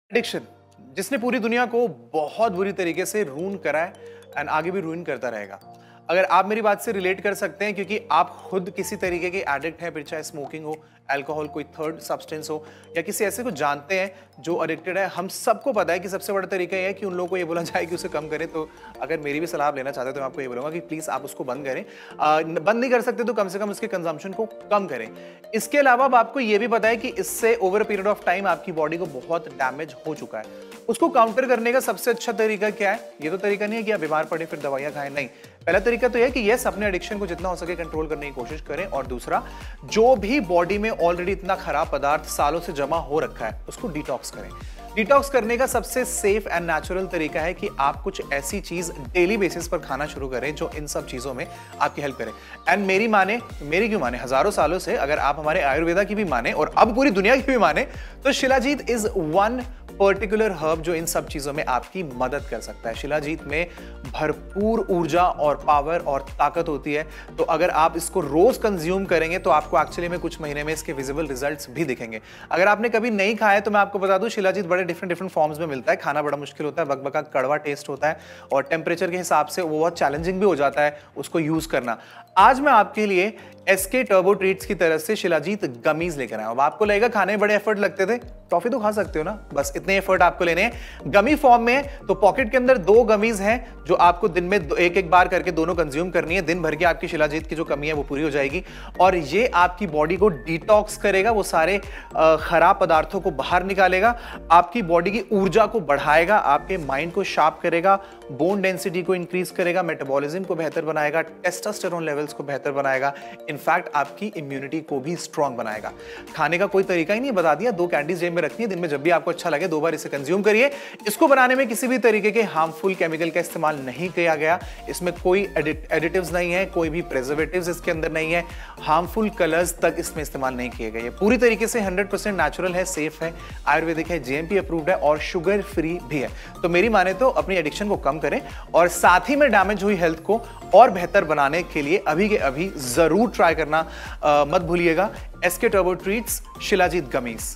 शिलाजीत गमीज़ से बॉडी डिटॉक्स: स्वास्थ्य और सौंदर्य को बढ़ाएं!
SK Turbo Treats Shilajit Gummies are Made with Shilajit
It is Rich in Fulvic Acid, a natural, simple way to fight signs of aging and improve the look and texture of your skin, and provides a strong immune system.
It is Sourced from the Higher Altitudes of the Himalayas region.
It is Purified to perfection with traditional Ayurvedic Process
Shilajit Gummies are:
1. Sugar-Free
2. Gluten Free
3.  GMO-Free
4.  Gelatin Free and
5.   Vegetarian
     With
1. No added preservatives
2. No added color
3. No bitter taste

These are Processed in a GMP-certified facility
And Support vigor and stamina.

एसके टर्बो ट्रीट्स शिलाजीत गमीज़ को शिलाजीत से बनाया जाता है, यह फुल्विक एसिड से भरपूर होता है, उम्र बढ़ने के संकेतों से लड़ने का एक नेचुरल और आसान तरीका है और आपकी त्वचा के रूप और बनावट में सुधार करता है, साथ ही इम्यून सिस्टम को मजबूत बनाता है। यह हिमालय क्षेत्र के सबसे ऊंची ऊंचाई से प्राप्त होता है और पारंपरिक आयुर्वेदिक प्रक्रिया से पूरी तरह से शुद्ध किया गया है।
शिलाजीत गमीज़ है
शुगर रहित
ग्लुटन रहित
GMO रहित
जेलेटिन रहित
शाकाहारी
कोई प्रीज़रवेटिव नही
कोई रंग नही
कड़वा टेस्ट नही

इन्हें एक GMP प्रमाणित फैसिलिटी में बनाया जाता है। य आपकी शक्ति और सहनशक्ति को बढाती है।